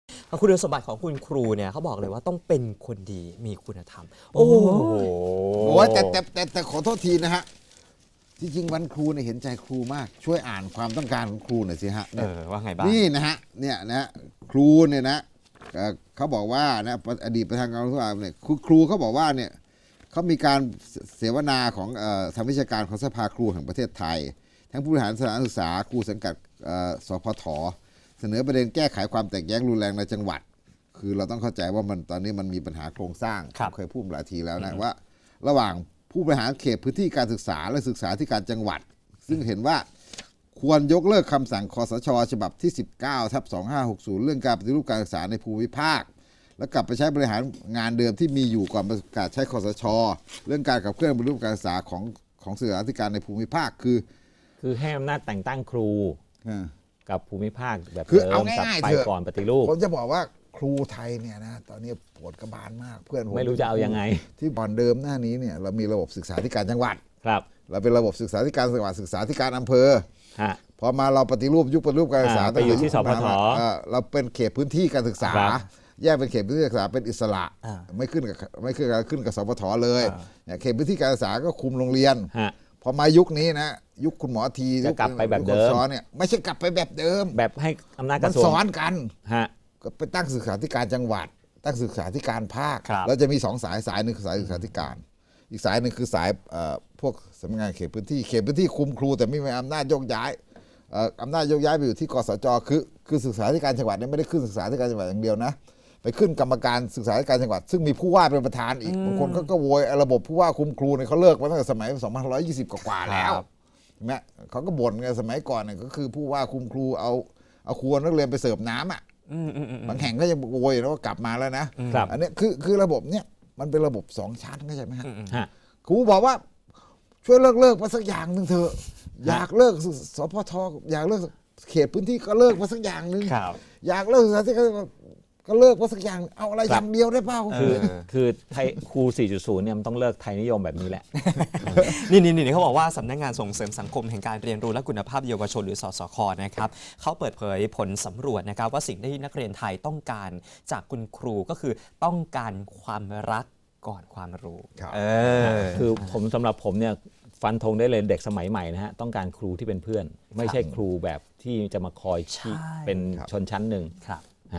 Wake Up News - ครูไทย 4.0 คือครู ปชต.ไทยนิยมนั่นแหละ
ครูไทยยุค 4.0 ต้องเป็นคนดี มีคุณธรรมและต้องเลิกไทยนิยม แต่สิ่งที่'นักเรียนไทย' ต้องการอยากได้ความใส่ใจเป็นรายบุคคลจากคุณครูอย่างจริงจังมากกว่านี้ และอยากได้ความใส่ใจอย่างเท่าเทียม ไม่ลำเอียงไปรักเด็กที่เรียนเก่งมากกว่า โดยยอมรับว่า ครอบครัวให้เวลาใส่ใจกับการเรียนของเด็กๆ น้อยไป นอกจากนี้ เด็กๆ ยังต้องการการยอมรับจากเพื่อนๆ สามารถพูดคุยปรึกษาปัญหา และช่วยติวหนังสือกันเองได้